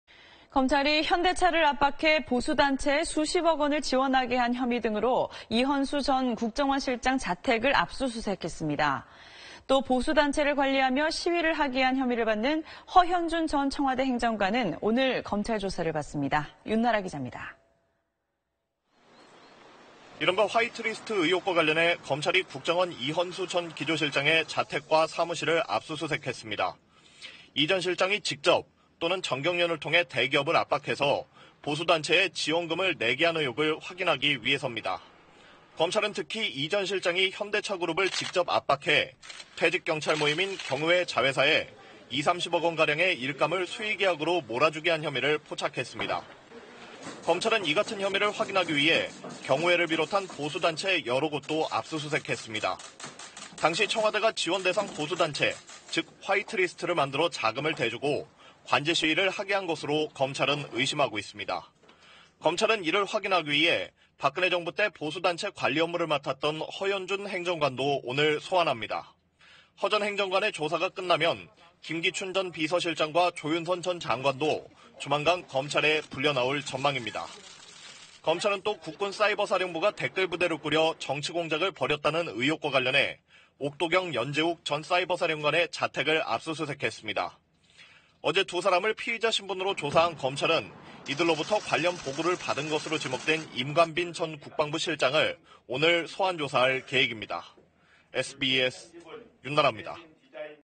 국정원 전 간부, 현대차 등 대기업 압박 / SBS
검찰이 현대차를 압박해 보수단체에 수십억 원을 지원하게 한 혐의 등으로 이헌수 전 국정원 실장 자택을 압수수색했습니다. 또 보수단체를 관리하며 시위를 하게 한 혐의를 받는 허현준 전 청와대 행정관은 오늘(12일) 검찰 조사를 받습니다.

윤나라 기자입니다.